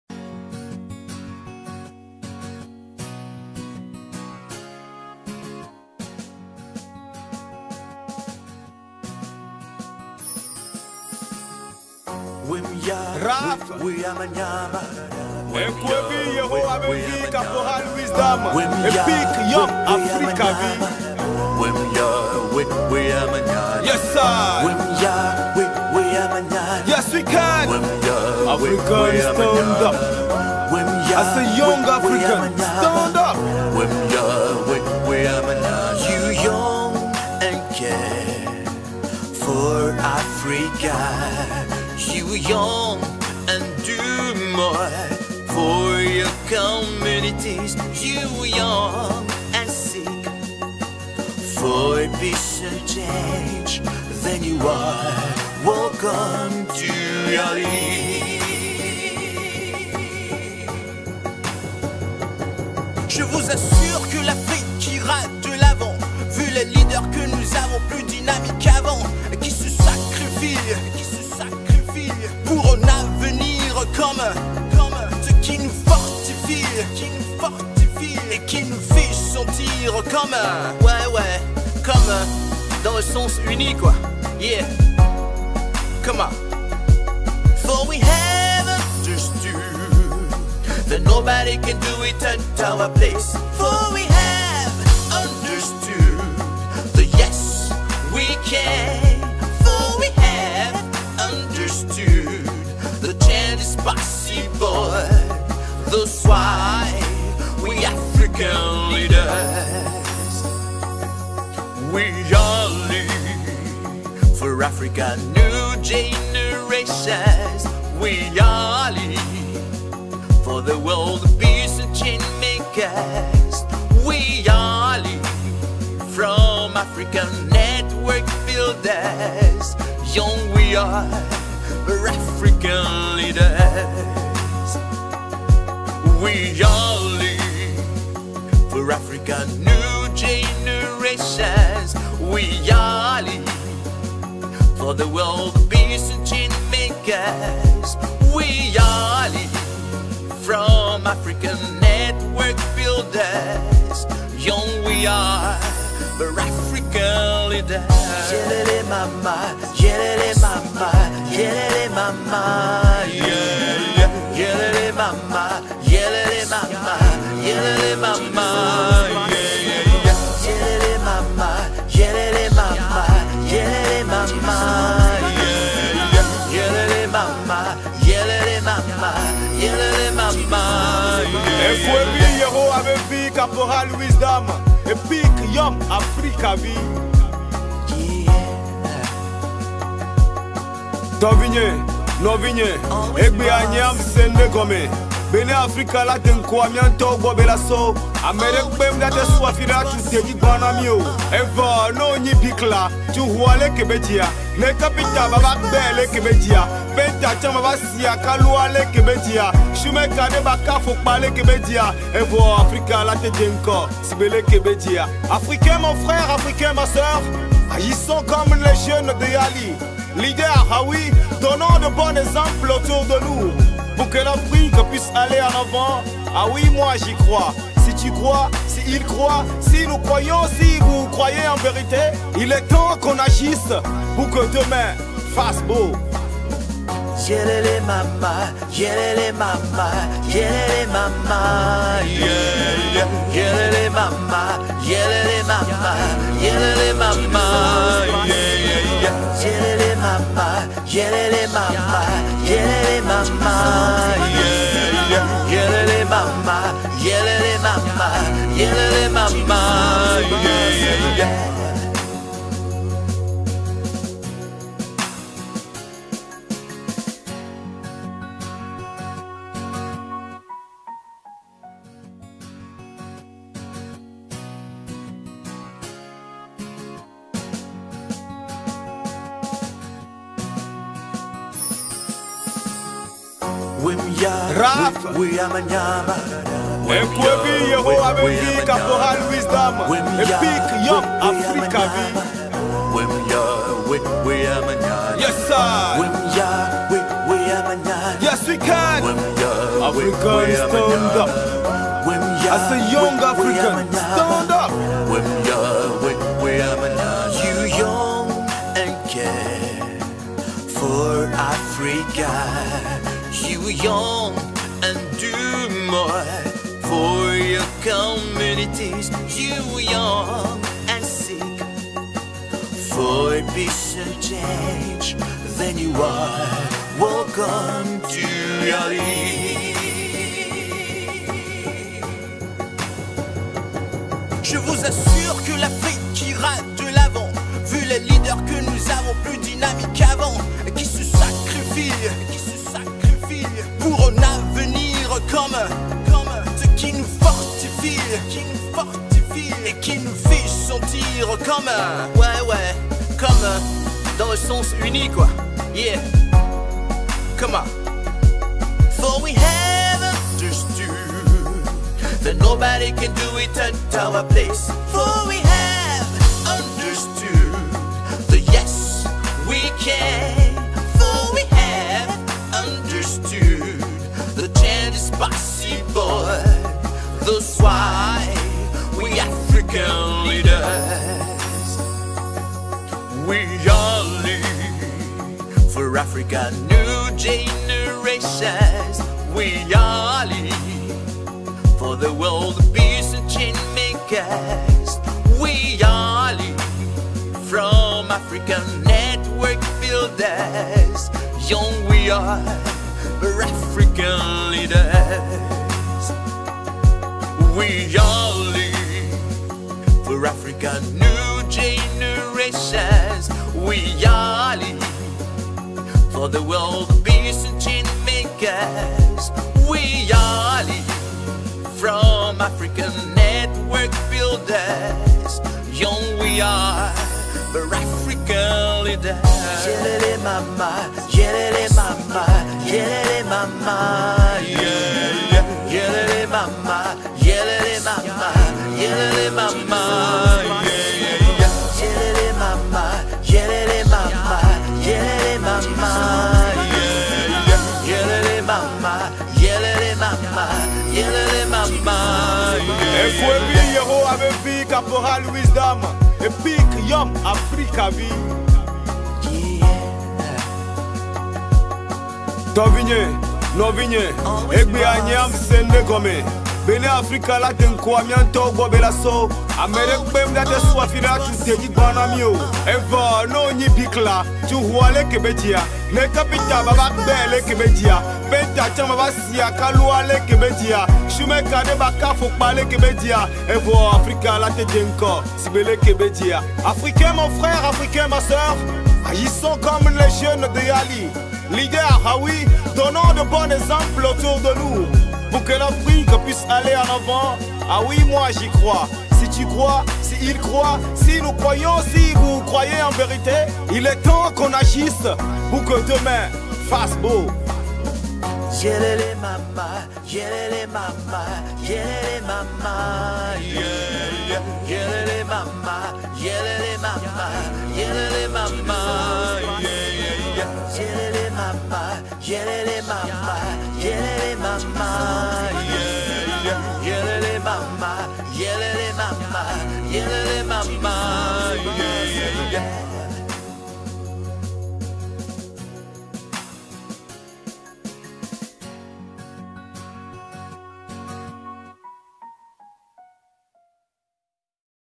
Réception en l'honneur des boursiers Togolais YALI Mandela Washington Fellow 2022
Réception en l'honneur des boursiers Togolais YALI Mandela Washington Fellow 2022 ce jeudi 15 septembre 2022 à la résidence de l'Ambassadeur des USA au Togo.